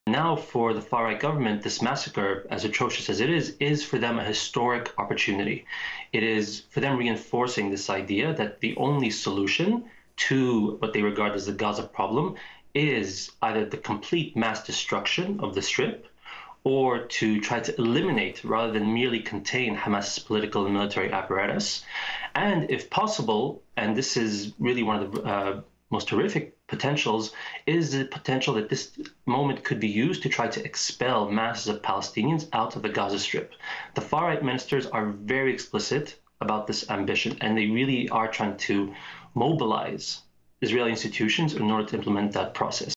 Israeli leaders see Hamas attack as "historic opportunity" to push far-right agenda on Gaza
Palestinian journalist and senior editor at _+972 Magazine_ Amjad Iraqi believes Hamas breaking through the military border between Gaza and Israel has shattered the belief that the occupation is sustainable.

Rather than end the blockade and apartheid regime, Iraqi suspects far-right leaders led by Prime Minister Benjamin Netanyahu will try to use Saturday's attack by Hamas to justify forcing out Palestinians or completely destroying Gaza.